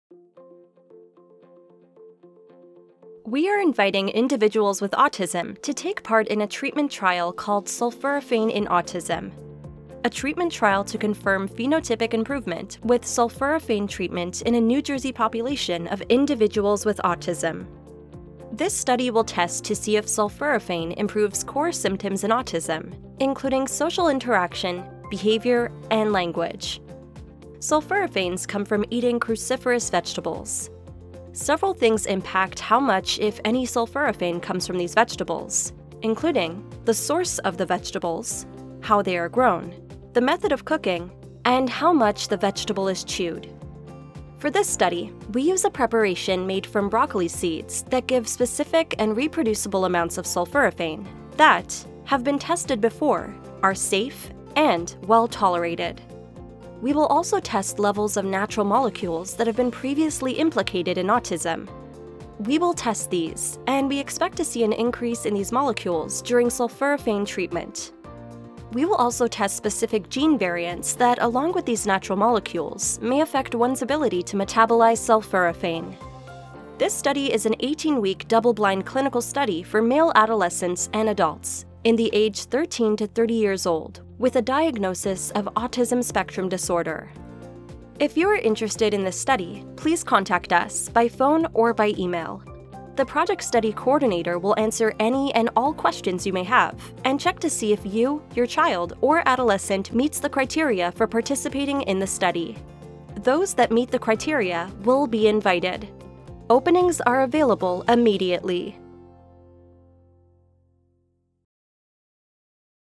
Clinical Trial in Autism Spectrum Disorder using Sulforaphane: Call for Research Participants
Sulforaphane in Autism. A treatment trial to confirm phenotypic improvement with sulforaphane treatment in an NJ population of individuals with Autism. Sulforaphane is a compound naturally found in cruciferous vegetables such as broccoli, kale, and cauliflower.